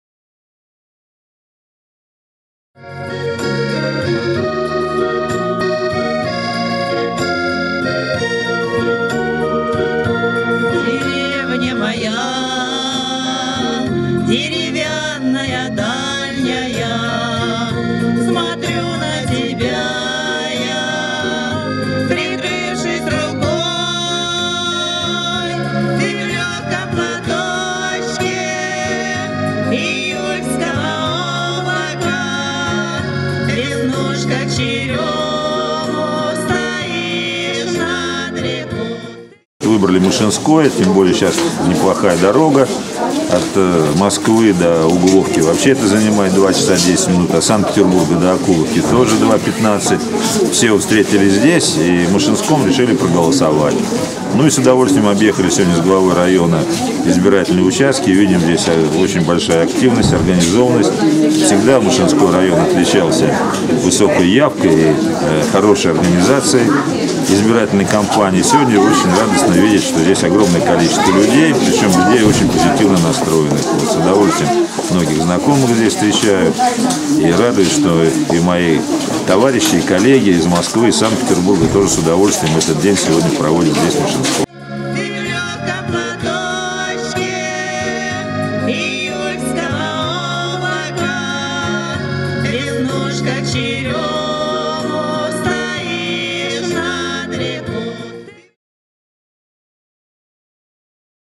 Экс-губернатор Митин проголосовал в Новгородской области
Выборы в Новгородской области. Голосует Митин